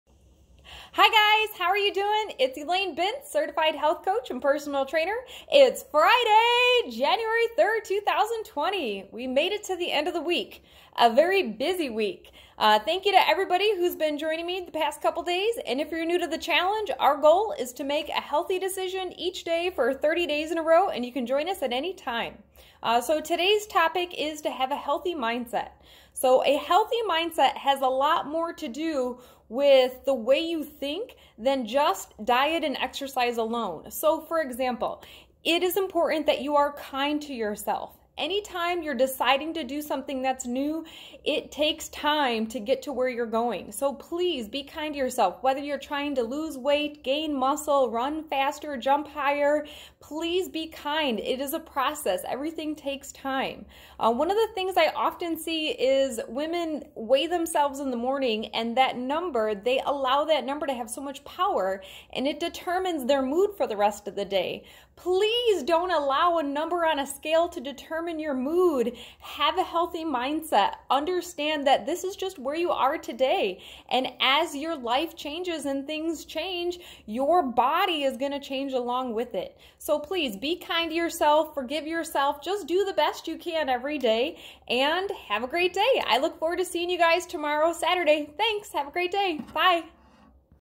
A healthy mindset w/Wellness Coach Elaine Bintz
Have a healthy mindset with Certified Health Coach & Personal Trainer, Elaine Bintz.